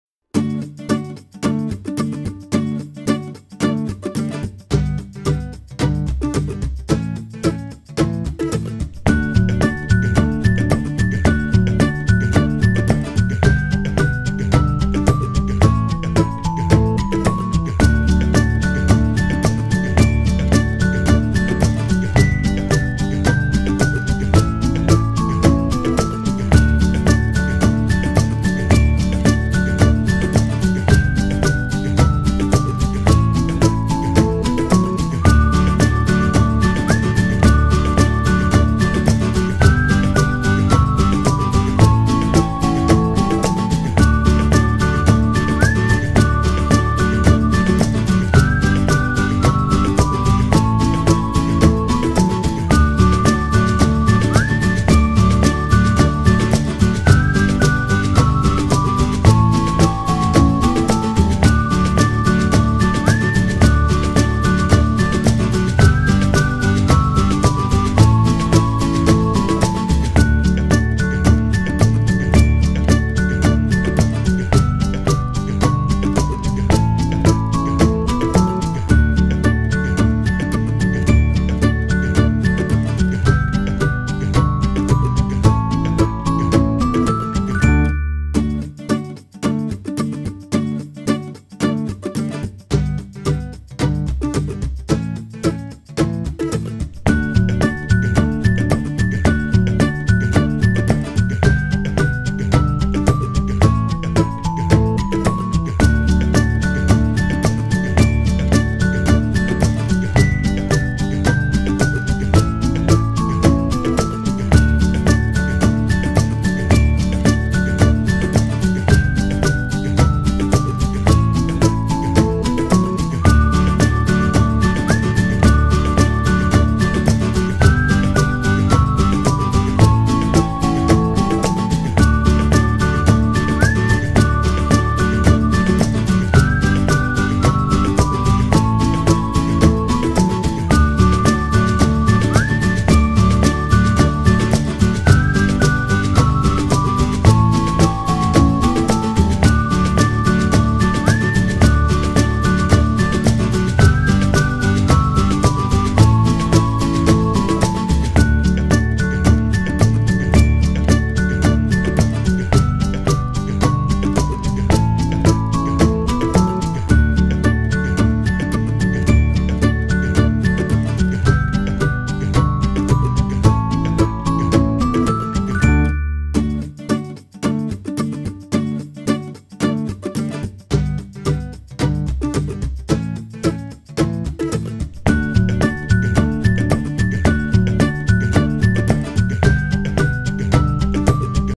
DIY 3d snowflake : how to make paper snowflake - snowflake making for Christmas / cool and creative
In this video you will see a Craft idea for Christmas, in which you learn how to make snowflake or ornament for Christmas. Hope you will like it and share it